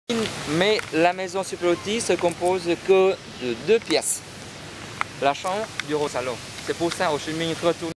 Hanoï - La Maison d'Ho Chi Minh 2
Elle est constituée de trois pièces: une chambre, un salon et un bureau.
C'est une maison humble. Ho Chi Minh n'aimait pas les décors ostentatoires.
Il ne voulait pas se servir de son poste de président du Vietnam du Nord pour vivre dans le luxe.